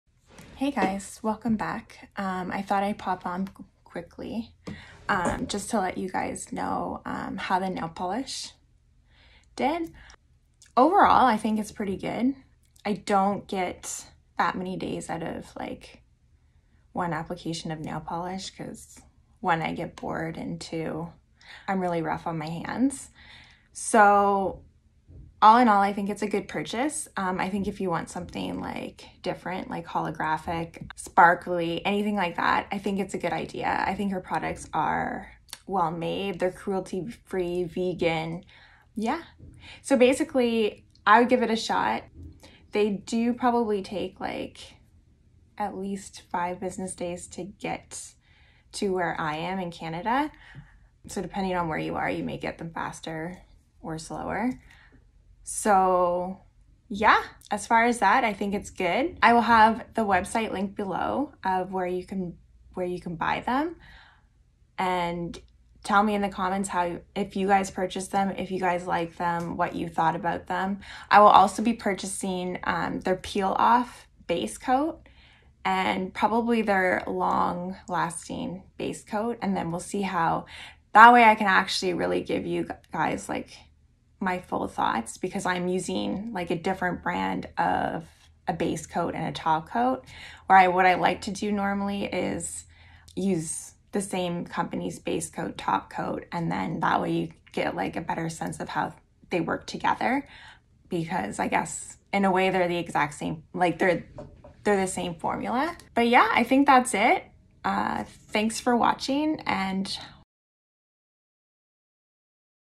Holo Taco review 2 days after
Hi guys, welcome back.  I wanted to include my overall thoughts of the new Holo Taco Rainbow Nail Polish as promised in my last video.  I hope you guys check out the website and consider purchasing these one of a kind polishes...you definitely won't be disappointed.  I have just received in the mail the peel off base coat and some of her other product, if you guys are interested in another review please let me know down in the comments.  Stay holo.

***New items that just launched on their website is their one year anniversary collection. HAPPY 1st BIRTHDAY HOLO TACO

Makeup used in this video:
Base Face:
***All prices in CAD unless specified***
* Face:
    * Milk Makeup Hydro Grip Primer $40.00
    * Maybeline Matte and Poreless Fit Me Foundation (in the shade Porcelain $8.96
    * NYX Can’t Stop Won’t stop Concealer (in the shade pale) $12.00
    * Hour Glass Ambient Lightening Edit Face (Bronzer) $105
    * Jouer Cosmetics Blush Bouquet Dual Blush (in the colour Flirt - mixed both shades) $40.00
    * Pat McGrath Labs Sublime Perfection Blurring Under-Eye Setting Powder (in colour Light) $40.00
    * Laura Mercier Translucent Setting Powder (in the colour Translucent) $51.00
    * Pat McGrath Labs Sublime Skin Highlighter Trio $67.00
    * Physicians Formula Organic Wear Nutrient Mist Facial Spray $16.99
* Eyes:
    * ABH Norvina Pro Pigment Palette Vol. 3
* Lips:
    * Pat McGrath Labs BlitzTrance Lipstick (in the colour Lady Stardust - shimmer light rose mauve with champagne sheen) $50.00
    * Pat McGrath Labs Lust: Lip Gloss - Divine Rose Collection *Limited Edition - Exclusive* (in the colour Devine Rose - ultimate plum rose with reflective pearl) $37.00

***All products were purchased by me, with my own money.